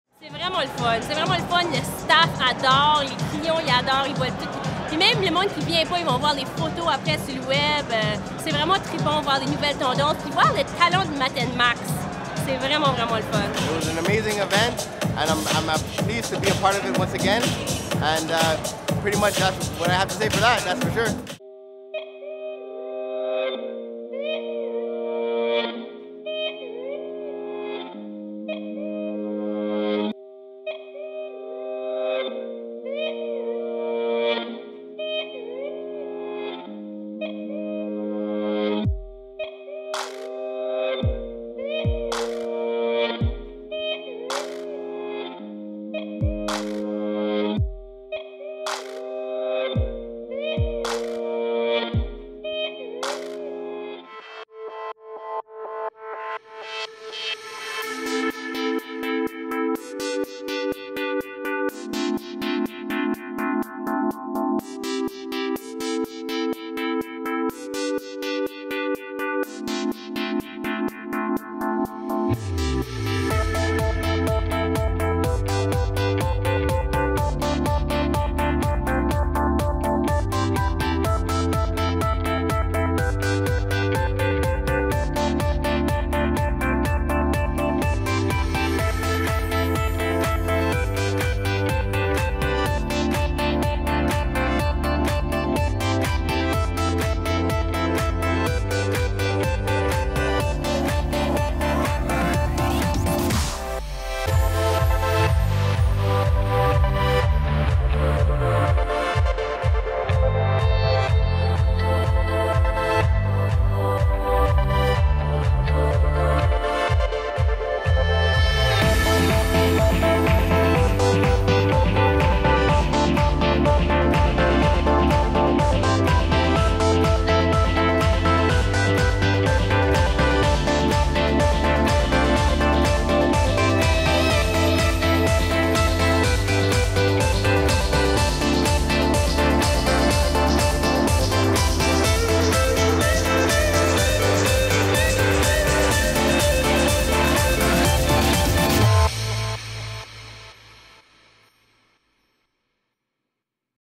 Événement Velours - Mat&Max PRO - 2014
Défilé Mat&Max PRO - 6 mai 2014

Découvrez les tendances Printemps / Été 2014 lors de notre show annuel de coiffure présenté par l'équipe artistique de Mat&Max

Membres de l'équipe :
- Junior Hernandez - Mat&Co
- Yohan Flores - Max & Co
- Stéphanie Goguen - Max &Co
- Maya Poulin - Max&Co
- Régina Fofana - Mat&Max Centre Eaton
- William Nd - Mat&Max Centre Eaton